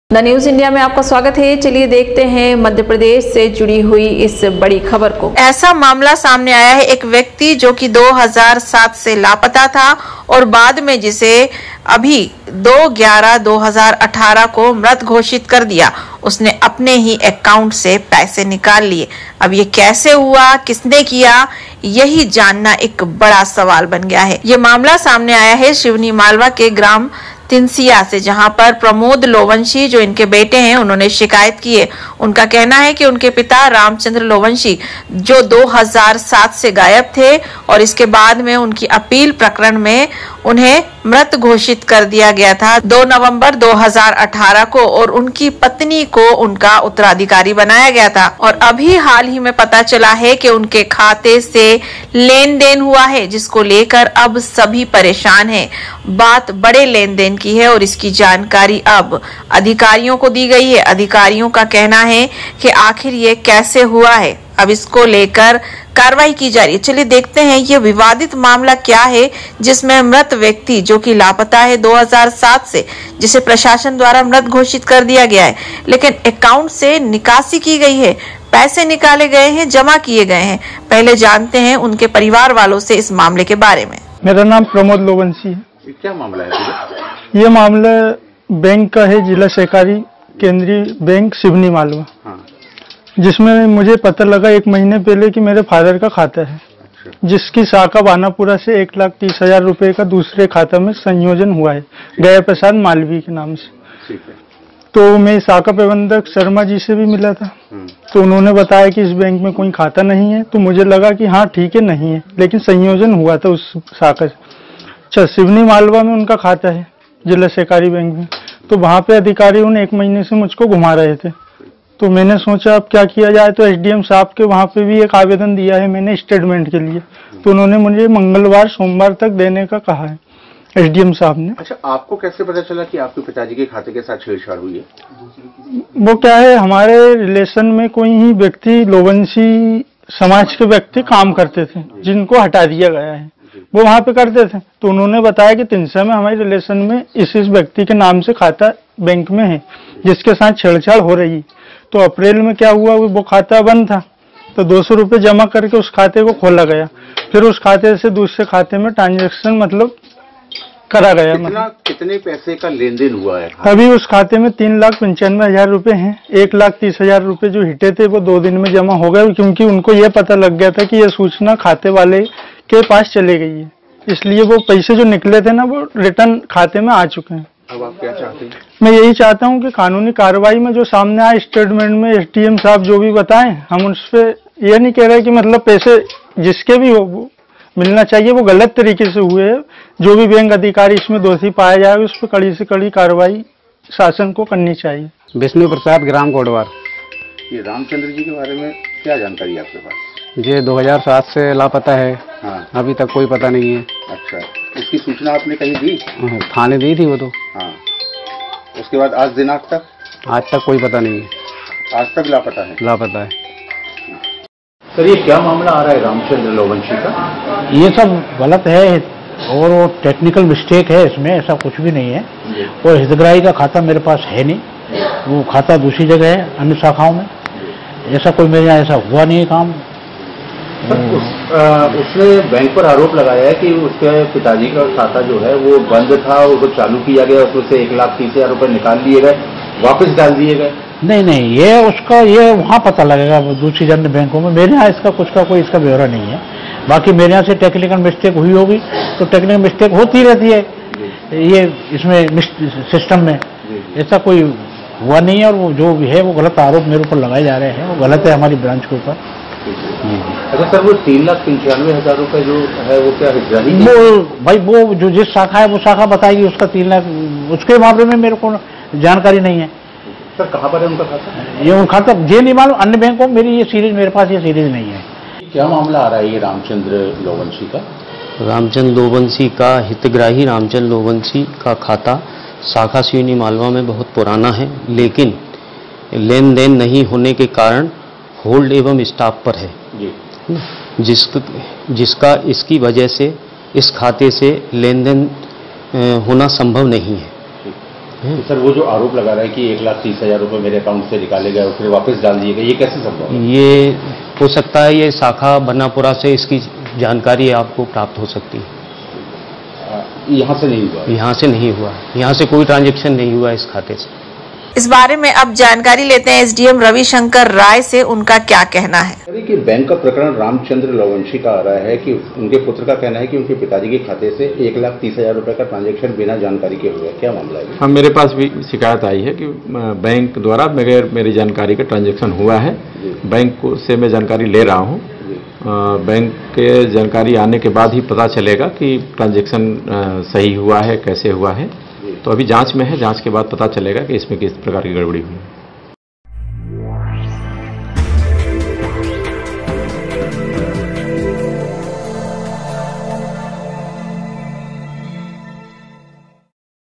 मृत व्यक्ति की आत्मा ने निकाले बैंक से 1लाख 30 हजार रुपये
सिवनी मालवा के ग्राम तीनसिया का सामने आया है ,इस बात की शिकायत प्रमोद लोवंशी ने एक शिकायती आवेदन के माध्यम से उच्च अधिकारियों को शिकायत की है।जिसमें उसने बताया कि उसके पिताजी  रामचंद्रर लोवंशी पिता छोटेलाल लोवंशी निवासी ग्राम तिनस्या जो कि सन 2007 से लापता है जिसका न्यायालय श्रीमान जिला होशंगाबाद मे अपील प्रकरण क्रमांक आरसीए 12/18 में माननीय न्यायालय द्वारा 2/11/2018 को रामचंद्रर को विधिक मृत घोषित  किया गया, Follow us on